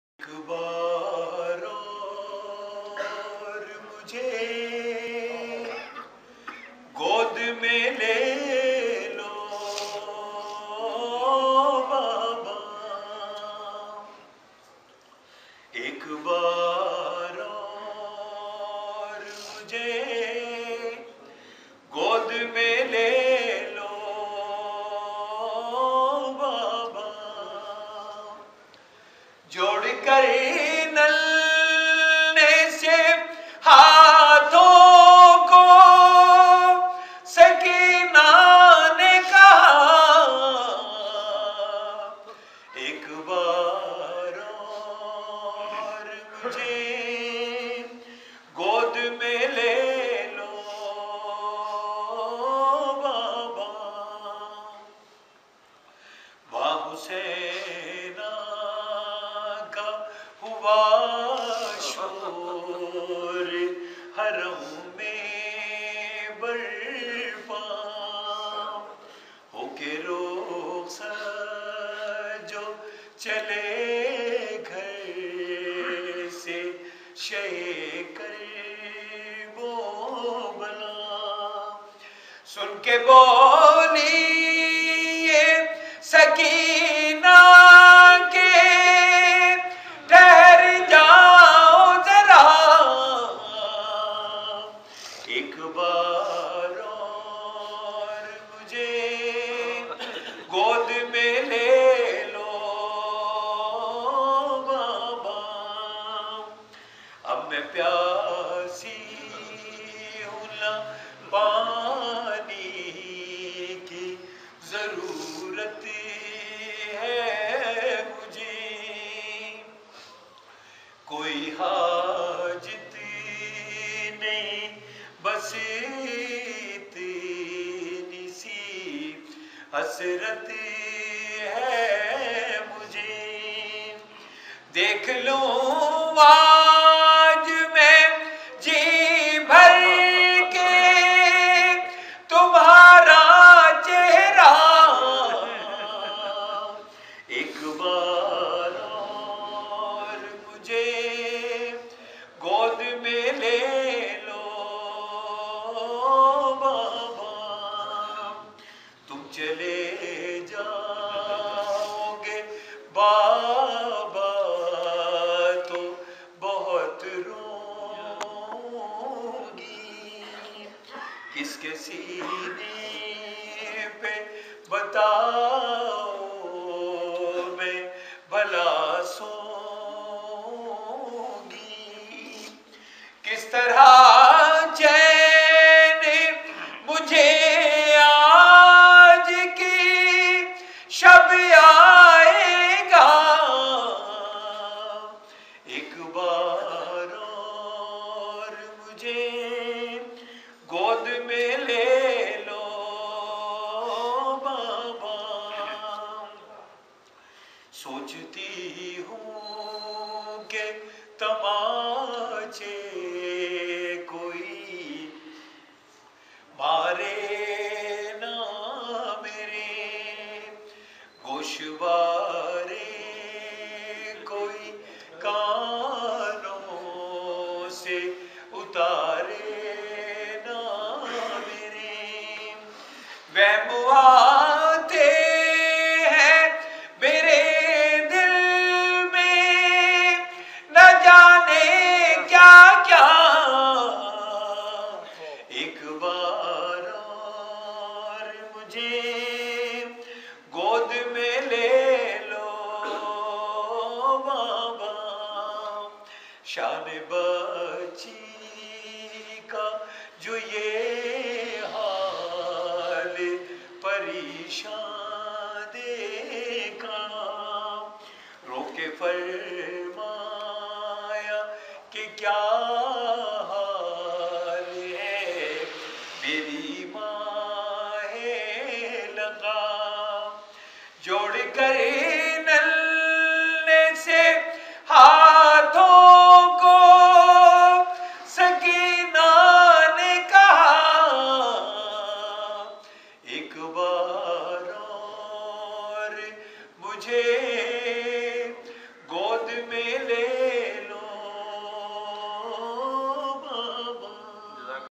Syed Raza Kazmi 030313-2 Majlis Soyem Syed Ibne Abbas Abidi Muhammadi Masjid Rawalpindi
Syed Raza Ali Kazmi
03 March 2013 Rabi us Sani 20-1434
Majlise Soyem Bri Syed Ibne Abbas Abidi
Maqam : Muhammadi Masjid new Katrian St Town Rawalpindi
Video Produced and Published by
Syed Shamshad Haider